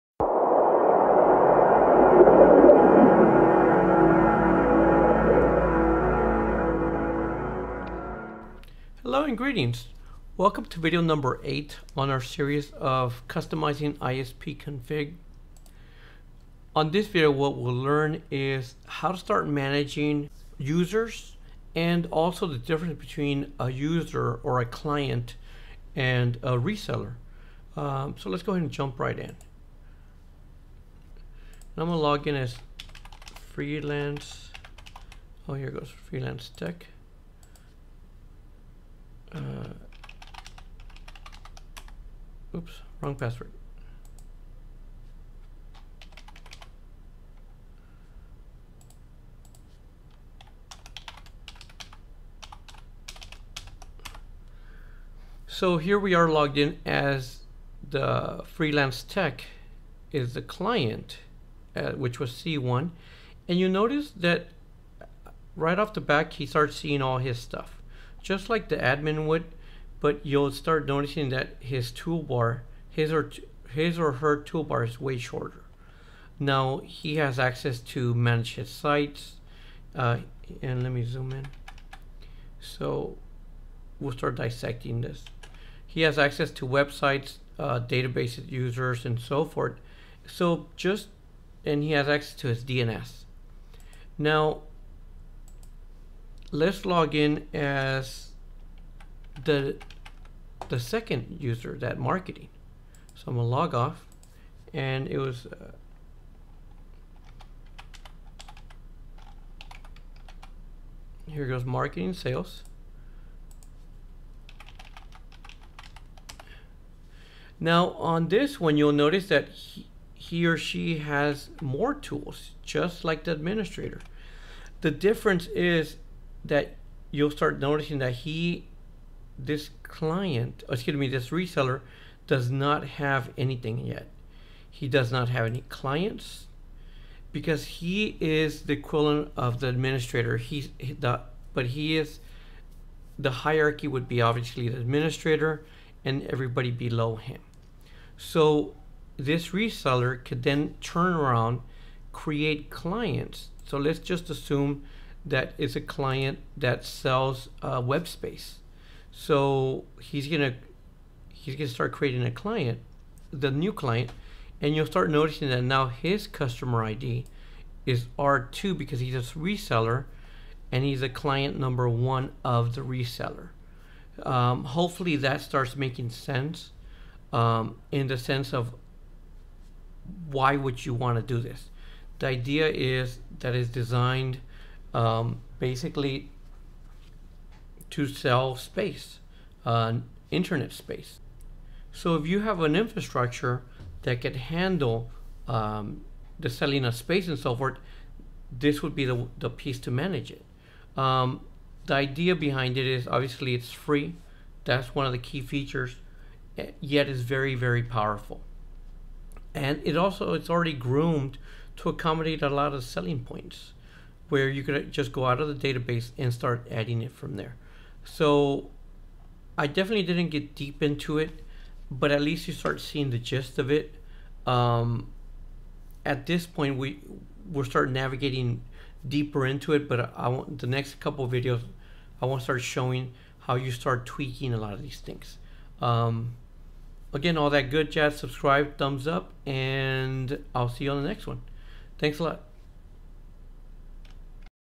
ISPconfig Tutorial Part 8 - Clients And Resellers Setup
On this tutorial I show how to create hosting Reseller and Clients by using ISPconfig 3.x.